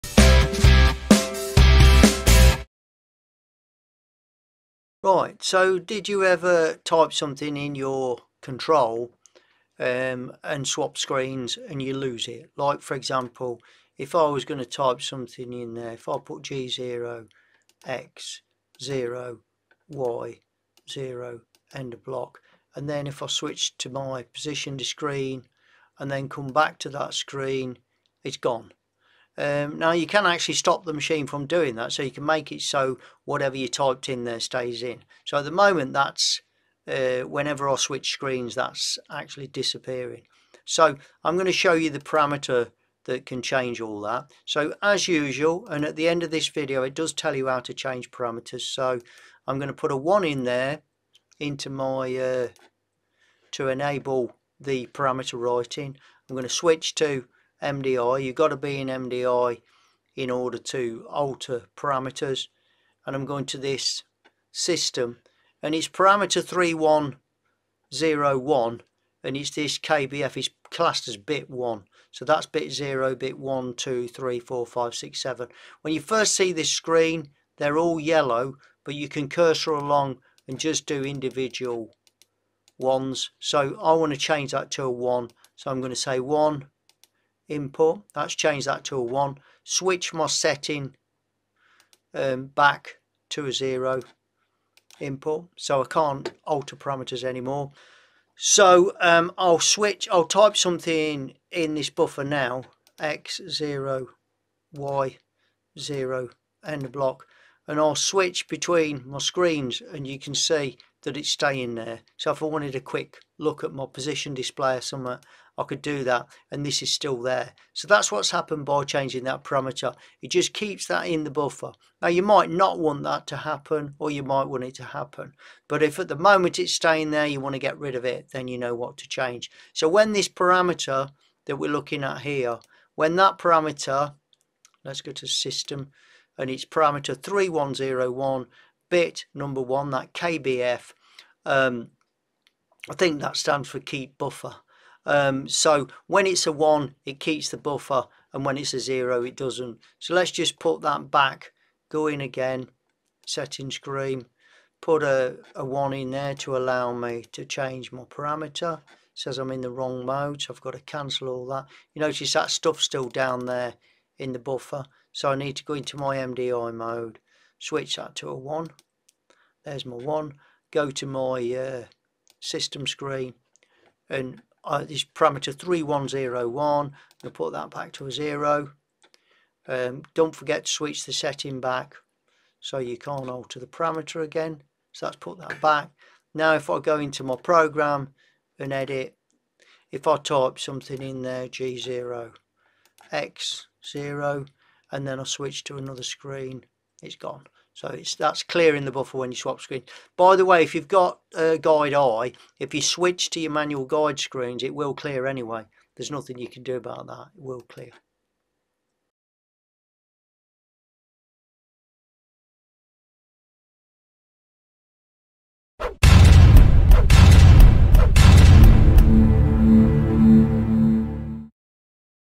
Keep what's Written in Buffer Fanuc Parameter 3101
When you type in commands on a Fanuc control it can be lost if you change screens.
By altering parameter 3101 you can change the way the machine behaves